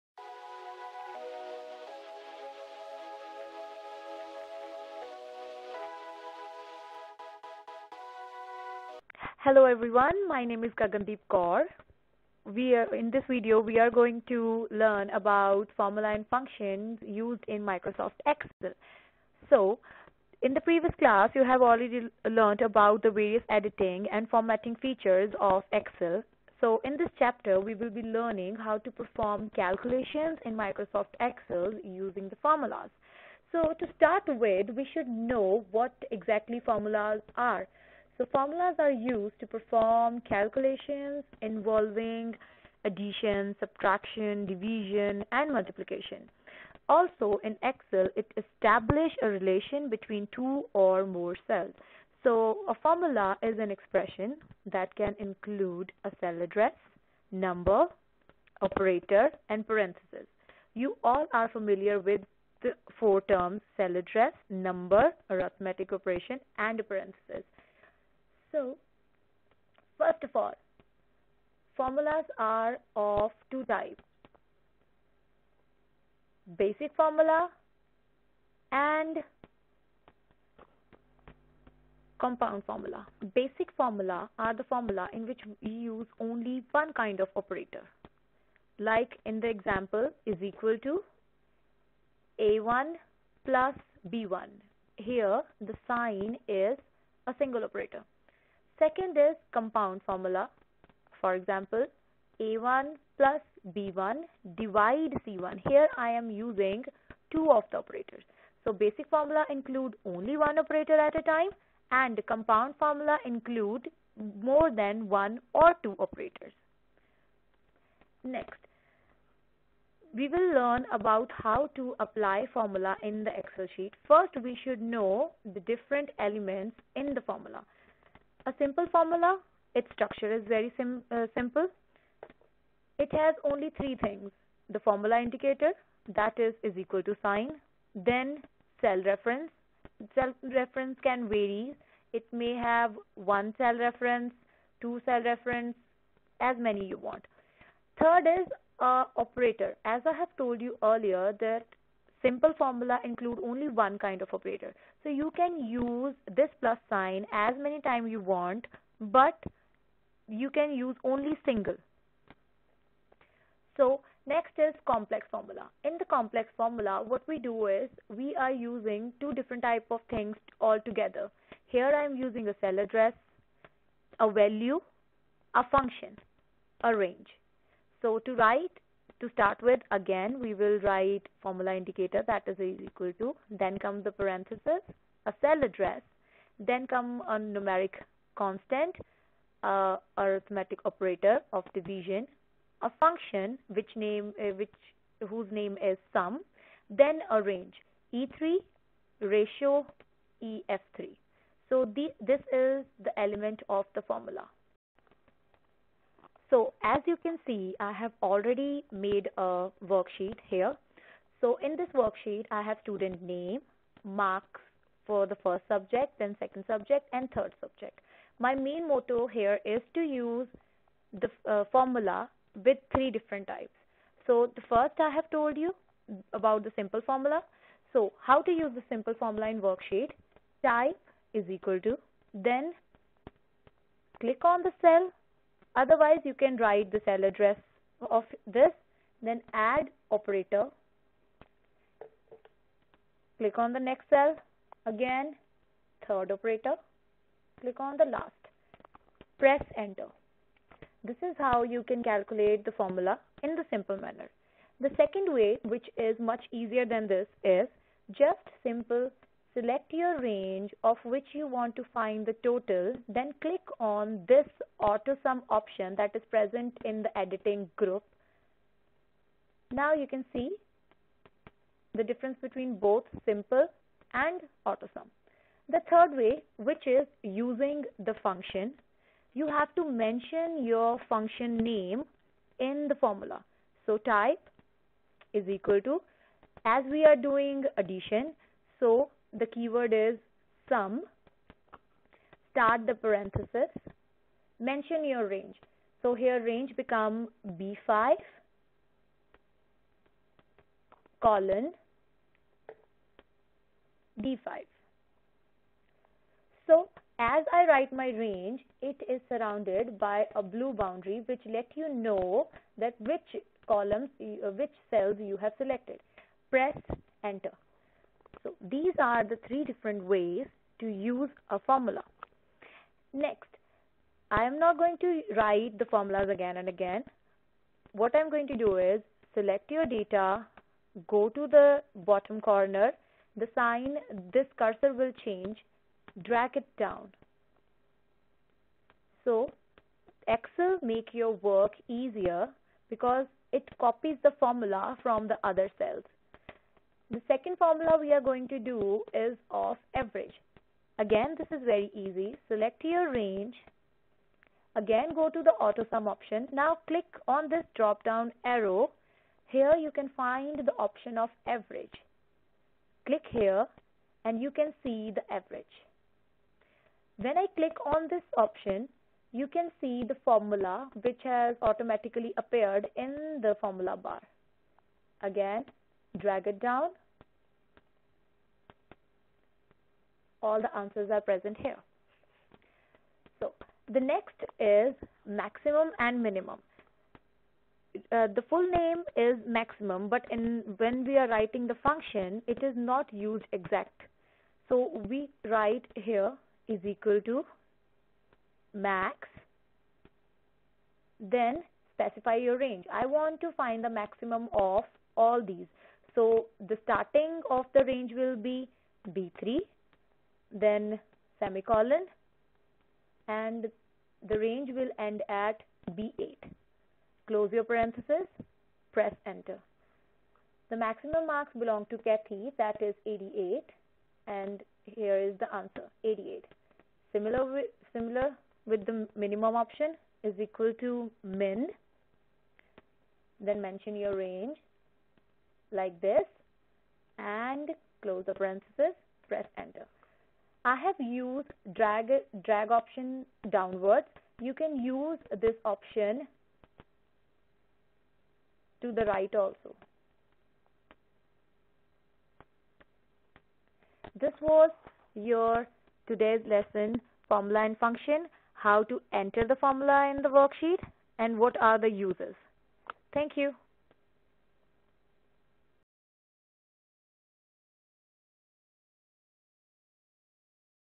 GRADE 7 || ICT || FORMULAS AND FUNCTION || PART 1
IN THIS VIDEO YOU WILL LEARN ABOUT FORMULAS AND ITS ELEMENTS, BASIC AND COMPOUND FORMULA